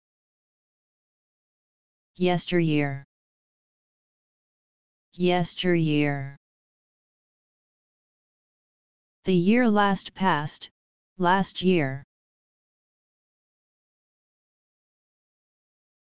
Yesteryear Meaning
How to pronounce Yesteryear in English?
What does Yesteryear mean?

Wordy Affair